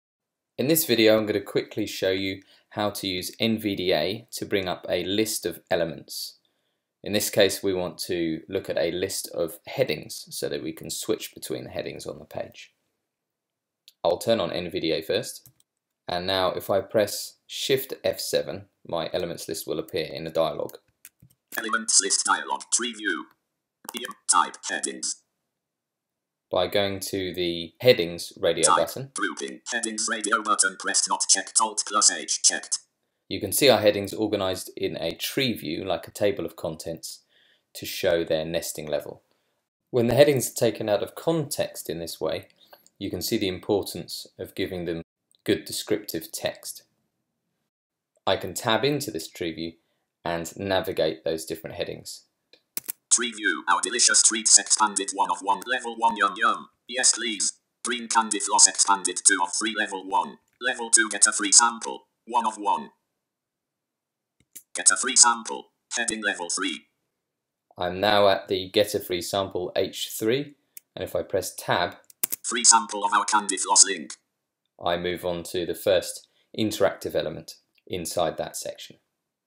The NVDA Heading Elements List
Exploring NVDA's elements list dialog to navigate between headings.